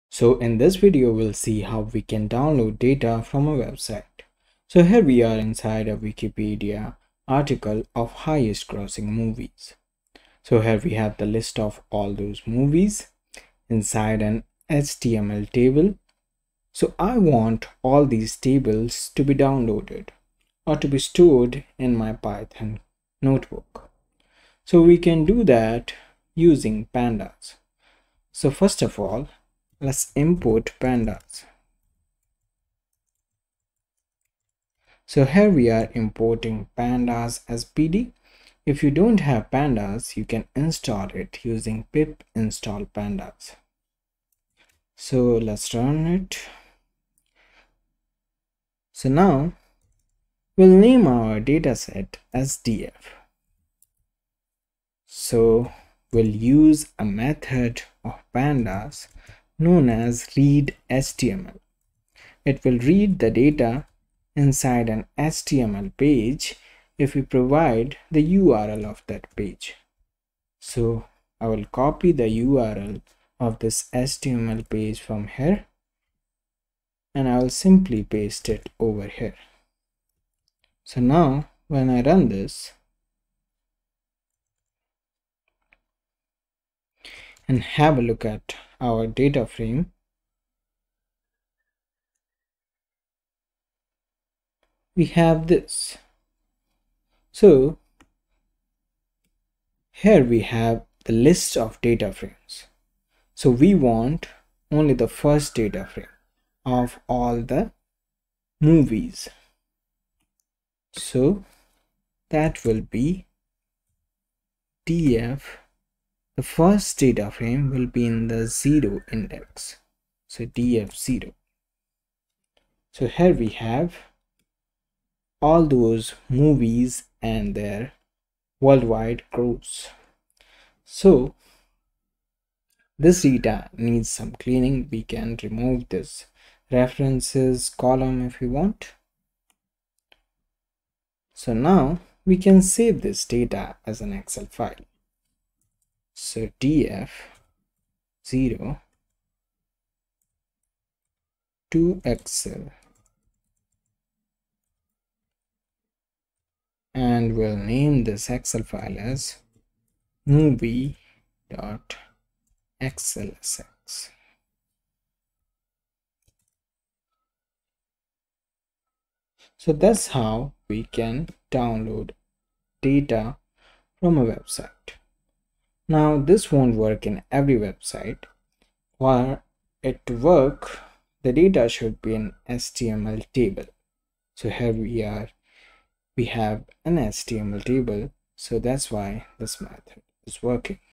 Downloading data from a website to excel using python.
In this tutorial, we are going to extract and download data from a website to excel using python pandas. We are going to extract html table data from a wikipedia page.

If you are trying data mining from website, this video might help. If you are trying to download data from a website using Python this video might help as well.